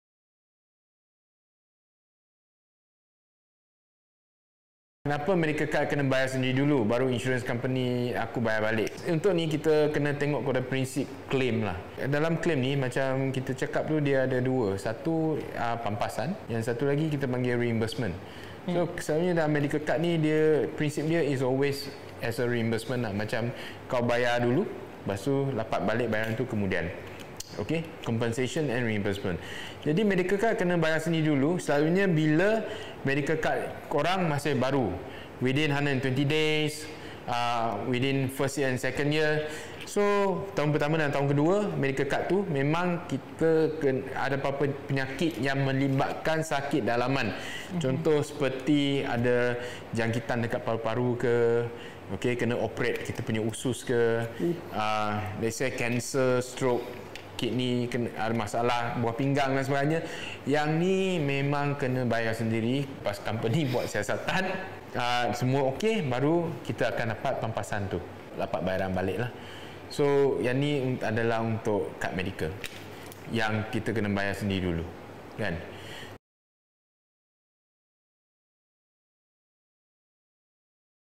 Mengapa kad medical nak pay and claim
Dalam tempoh pertama selepas mendapatkan medical card baru, terdapat tempoh tertentu di mana sesetengah penyakit atau rawatan mungkin tidak dilindungi sepenuhnya